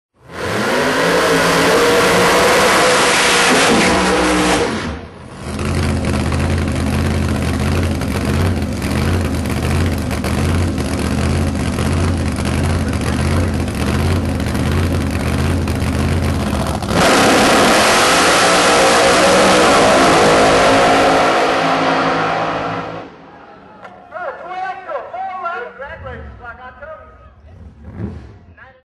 ProMod 4.11 Orangeburg, SC
Nice ProMod run... 4.11 to a 4.12 in the heat of the day at Orangeburg Dragstrip, SC.

Samsonperformance.com
Samson Performance
Sam's Frame and Alignment